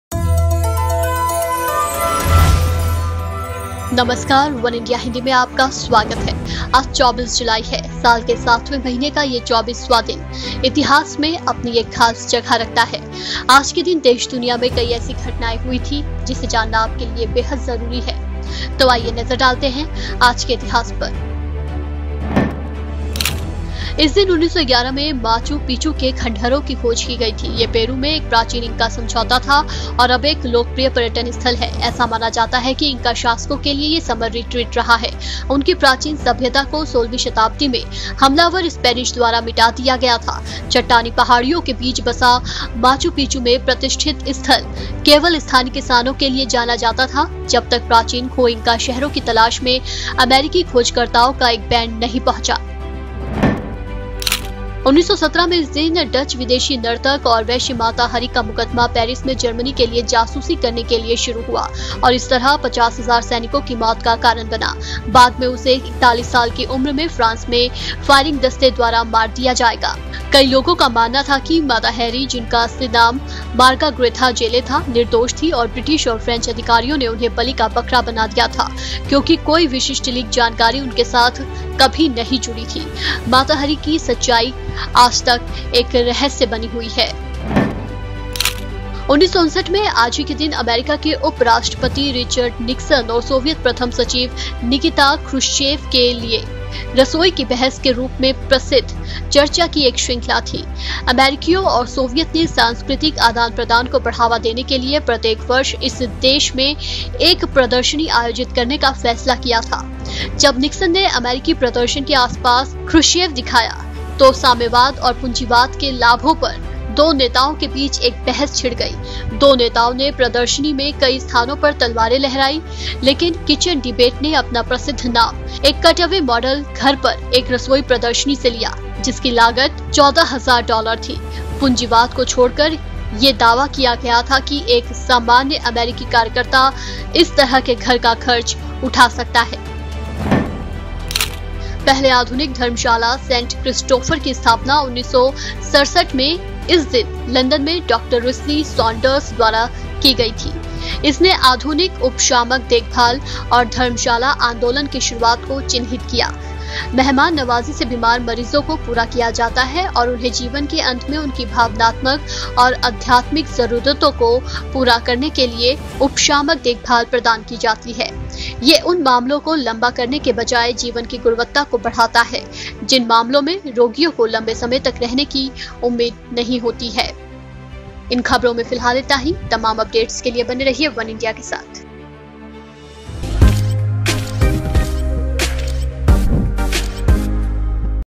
24 July History : देश और दुनिया के इतिहास में 24 जुलाई का है खास महत्व | वनइंडिया हिंदी
Today is  July17. It occupies a special place in the seventeenth day of the seventh month of the year. On this day, there were many such incidents in the country and the world, which is very important for you to know. So let's have a look at today's history

आज 24 जुलाई है। साल के सातवें महीने का ये चौबीसवां दिन में अपनी एक खास जगह रखता है। आज के दिन देश-दुनिया में कई ऐसी घटनाएं हुई थीं जिसे जानना आपके लिए बेहद ज़रूरी है। तो आइए नज़र डालते हैं आज के इतिहास पर।